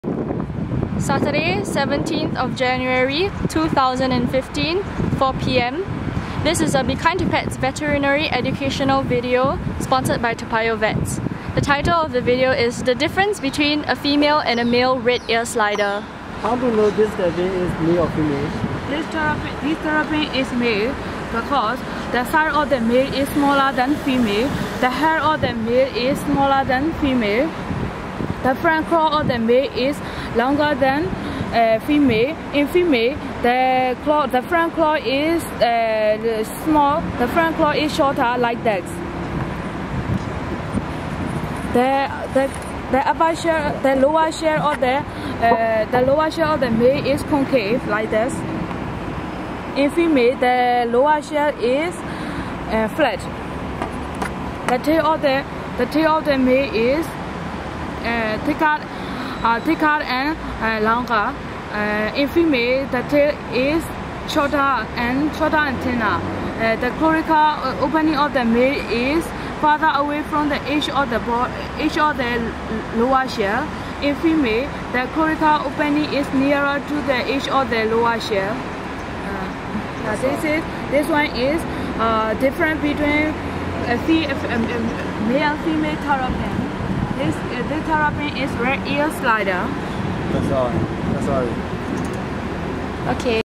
Differences between male and female red-eared slider - May Pt 2
Sat 17 Jan 2015  4 pm. Teaching veterinary assistant from Myanmar how to tell the differences